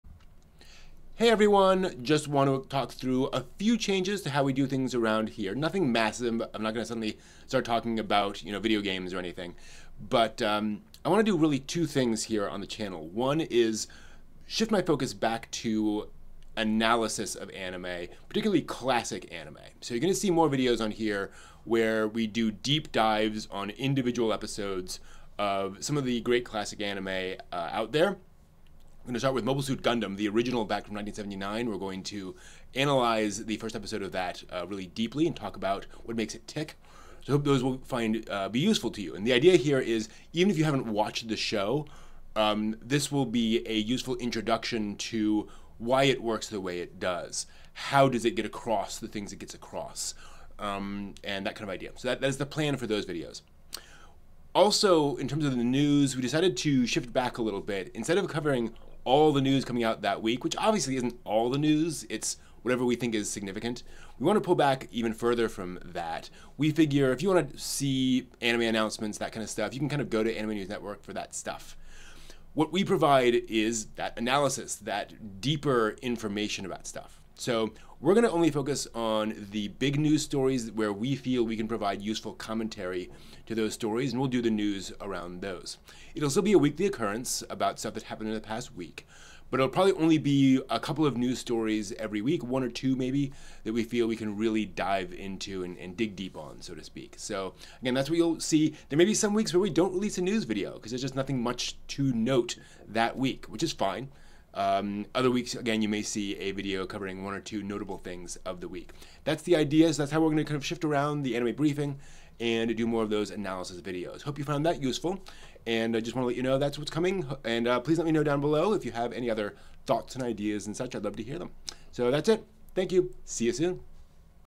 State of the channel: more analysis, less news (but still news)
We're going to shift the content of the channel back towards in-depth analysis of classic anime, as well as only covering the news items that we can provide useful commentary and context on. That's it!

We dig deeper into anime history and culture on our Discord